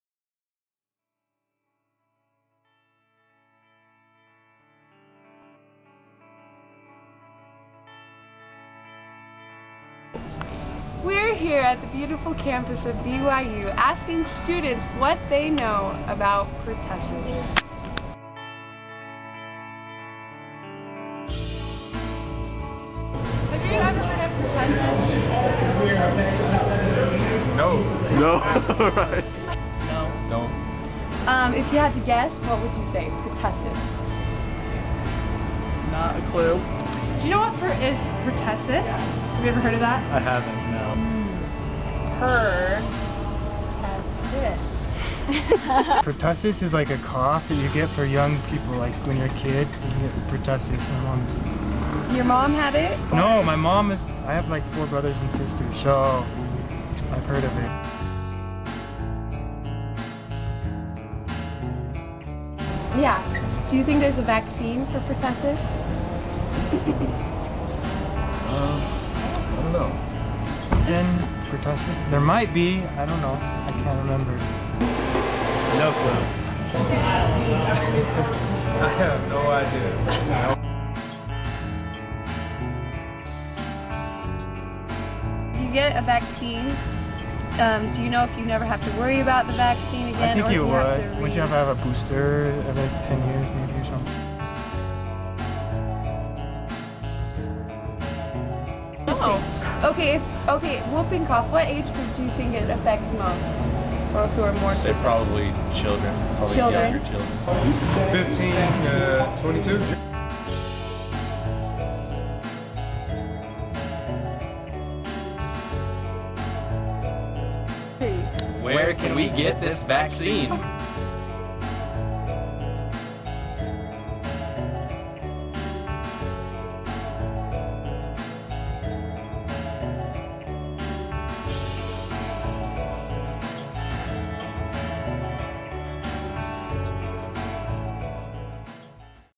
Pertussis Information
Pertussis is affecting college students more than ever. Watch this video to learn more about the disease and how to prevent it.